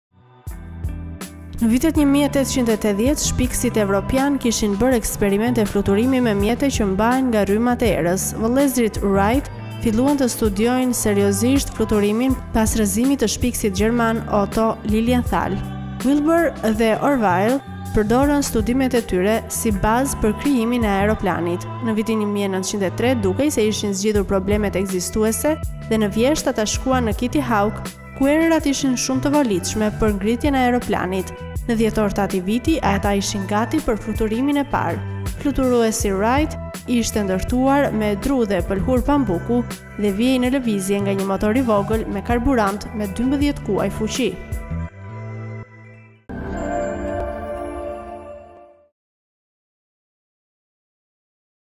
Aeroplani i parë, si lindi ideja për të?
Në vitet 1880 shpikësit evropianë kishin bërë eksperimente fluturimi me mjete që mbahen nga rrymat e erës. Vëllezërit Wright filluan të studiojnë seriozisht fluturimin pas rrëzimit të shpikësit gjerman Otto Lilienthal. Wilbur dhe Orville përdorën studimet e tij si bazë për krijimin e aeroplanit. Në vitin 1903 dukej se ishin zgjidhur problemet ekzistuese dhe në vjeshtë ata shkuan në Kitty Hawk ku erërat ishin shumë të volitshme për ngritjen e aeroplanit.